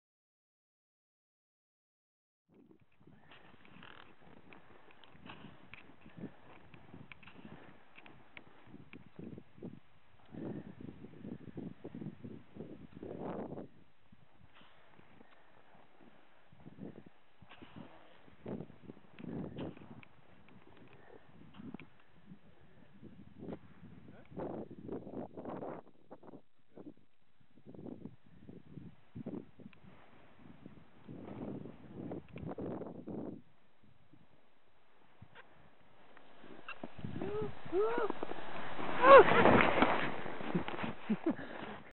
Skiing in the Norwegian mountains and forests - easy going when it's downhill - but look out behind.
Skiing in the freezing cold Norwegian mountains and forests. It's easy going when it's downhill and there's a trodden path - but look out behind, one of the first lessen of skiing is how to stop...